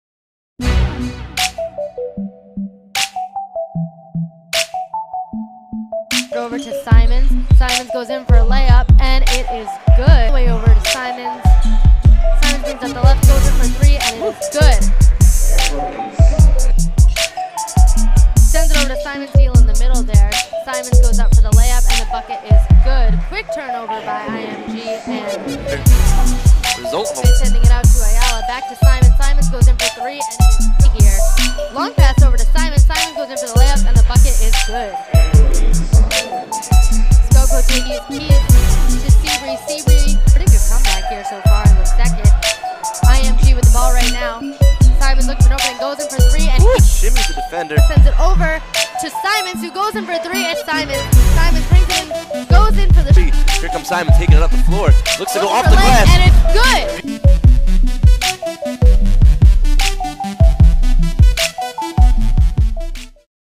Flo40 Anfernee Simons Dropped 29 in front of NBA Scouts This January 😱
In January, Anfernee Simons dropped 29 in front of NBA Scouts. Now, the Flo40 point guard has officially declared for the 2018 draft.

Check out the full 29-point performance from the 2018 Hoophall Classic